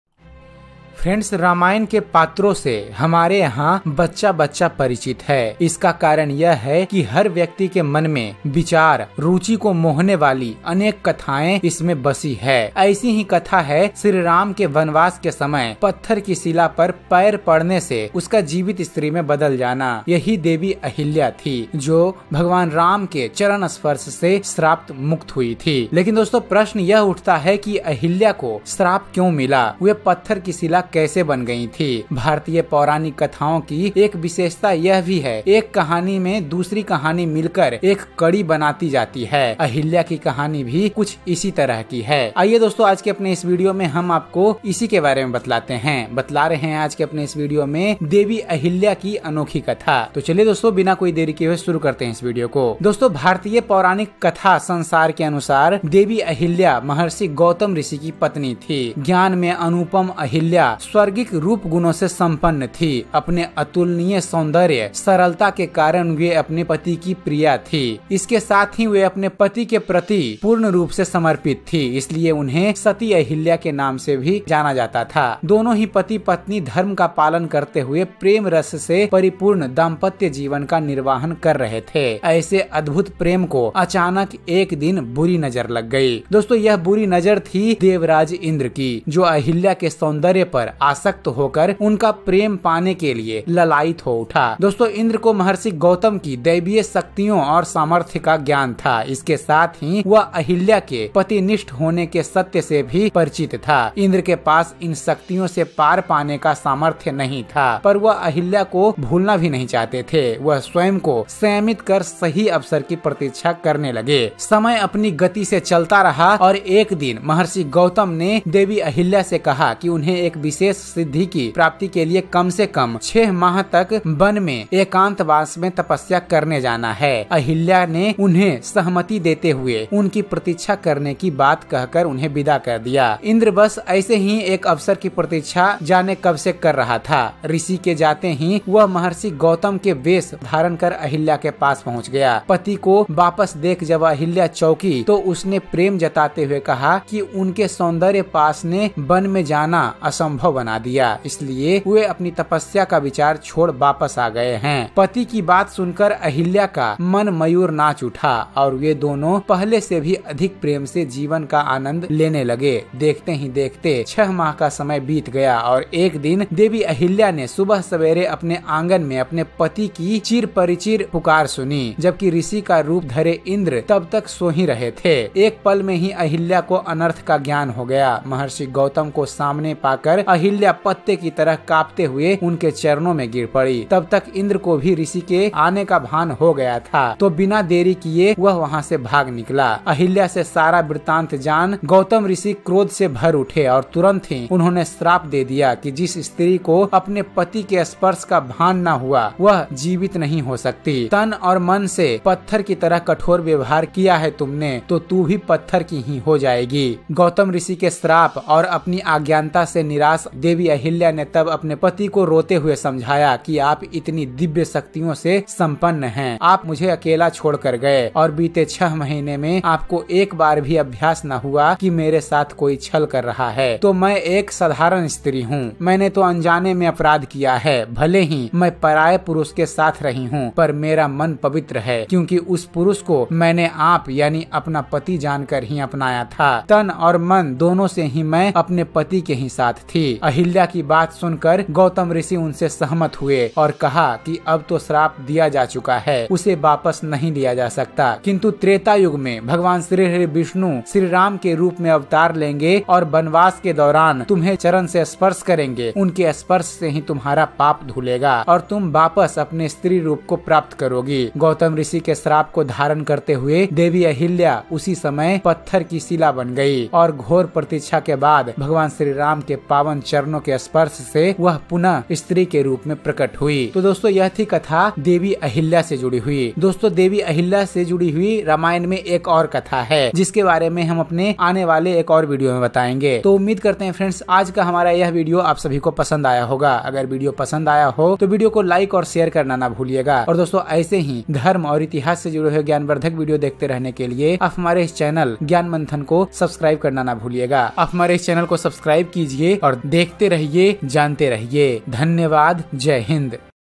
कौन थीं देवी अहिल्या और कैसे बन गई थीं वो पत्थर? Complete Story of Ahalya
Watch all Episode Of Ramyana,Mahabharata,and Krishna Leela